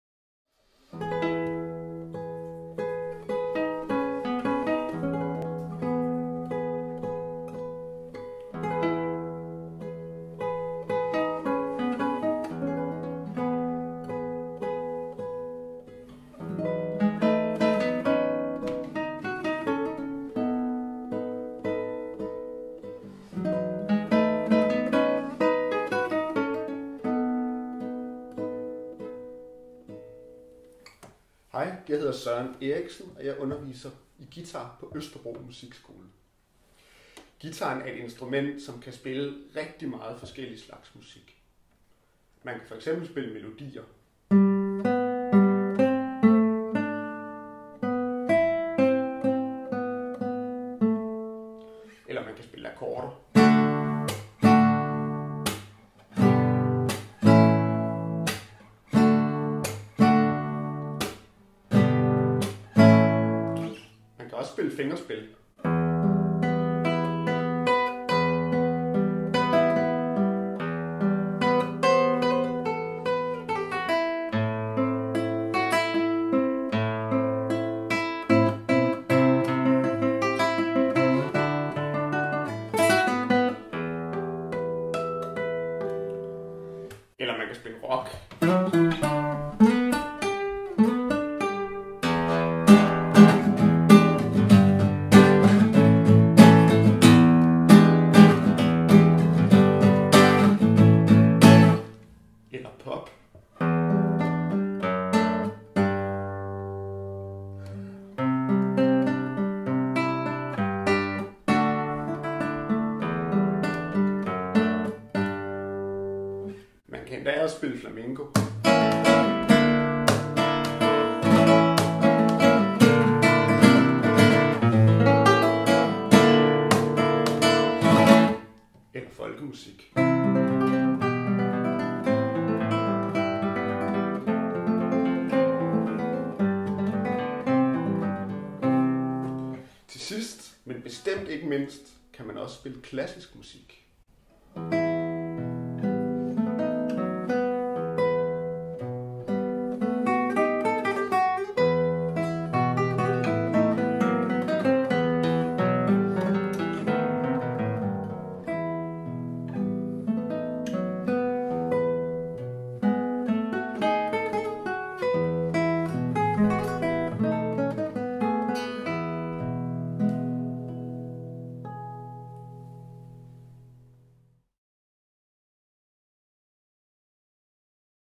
Østerbro Musikskole - Guitarlærer, Søren Eriksen
Videopræsentation af guitarlærer Søren Eriksen på Østerbro Musikskole.